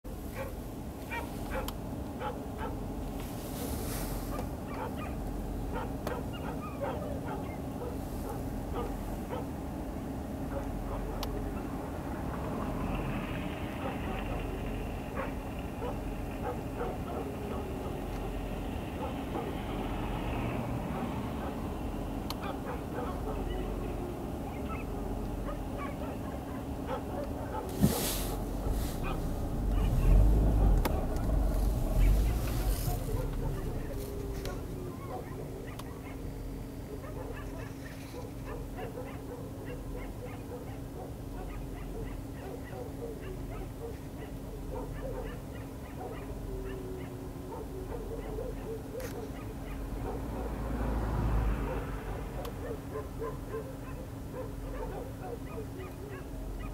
Homeless dogs howling from a frost like wolfs in Moscow
February 2010, Moscow, Russia